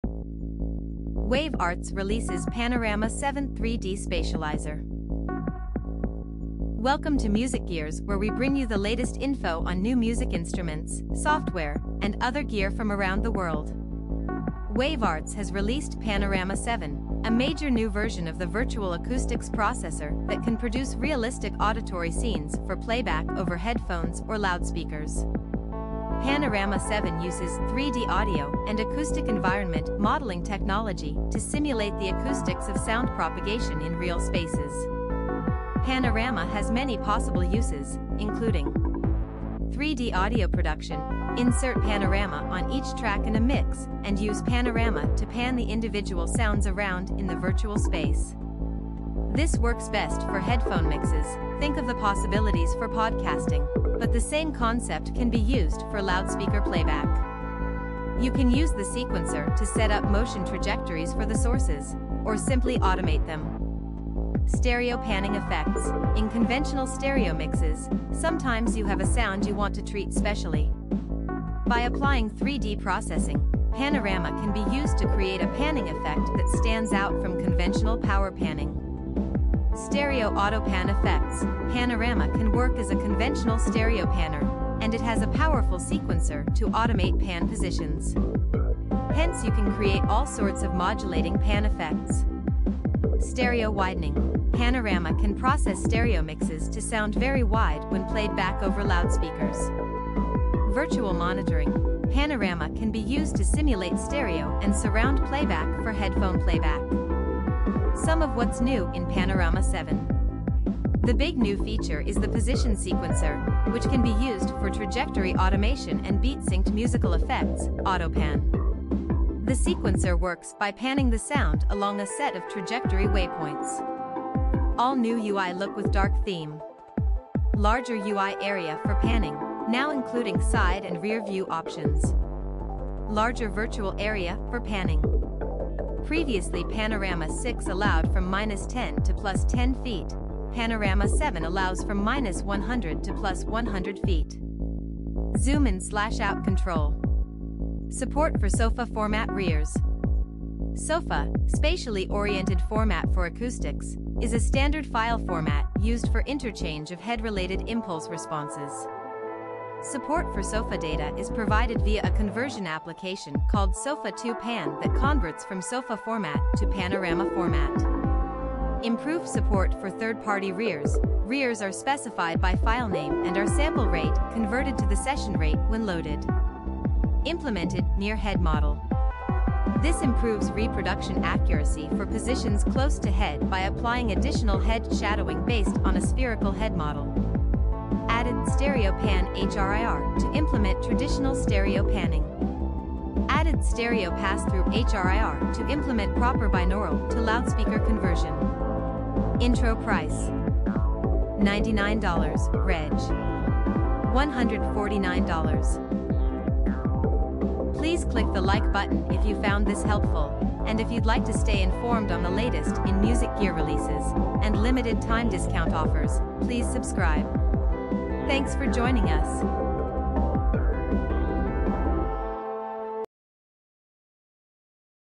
Wave Arts releases Panorama 7 - 3D Spatializer
Wave Arts has released Panorama 7, a major new version of the virtual acoustics processor that can produce realistic auditory scenes for playback over headphones or loudspeakers. Panorama 7 uses 3-D audio and acoustic environment modeling technology to simulate the acoustics of sound propagation in real spaces.

Panorama has many possible uses, including:

3D audio production: Insert Panorama on each track in a mix, and use Panorama to pan the individual sounds around in the virtual space. This works best for headphone mixes (think of the possibilities for podcasting), but the same concept can be used for loudspeaker playback. You can use the sequencer to set up motion trajectories for the sources, or simply automate them.
Stereo panning effects: In conventional stereo mixes, sometimes you have a sound you want to treat specially. By applying 3D processing, Panorama can be used to create a panning effect that stands out from conventional power panning.
Stereo "auto-pan" effects: Panorama can work as a conventional stereo panner, and it has a powerful sequencer to automate pan positions. Hence you can create all sorts of modulating pan effects.
Stereo widening: Panorama can process stereo mixes to sound very wide when played back over loudspeakers.
Virtual monitoring: Panorama can be used to simulate stereo and surround playback for headphone playback.
Some of what's new in Panorama 7:

The big new feature is the position sequencer, which can be used for trajectory automation and beat-synced musical effects (auto-pan). The sequencer works by panning the sound along a set of trajectory waypoints.
All new UI look with dark theme.
Larger UI area for panning, now including side and rear view options.
Larger virtual area for panning. Previously Panorama 6 allowed from -10 to +10 feet, Panorama 7 allows from -100 to +100 feet.
Zoom in/out control.
Support for SOFA format HRIRs. SOFA (Spatially Oriented Format for Acoustics) is a standard file format used for interchange of head-related impulse responses. Support for SOFA data is provided via a conversion application called Sofa2Pan that converts from SOFA format to Panorama format.
Improved support for third-party HRIRs: HRIRs are specified by filename and are sample rate converted to the session rate when loaded.
Implemented "Near Head Model". This improves reproduction accuracy for positions close to head by applying additional head shadowing based on a spherical head model.
Added "Stereo Pan" HRIR to implement traditional stereo panning.
Added "Stereo Passthru" HRIR to implement proper binaural to loudspeaker conversion.
Intro Price: $99 (Reg. $149)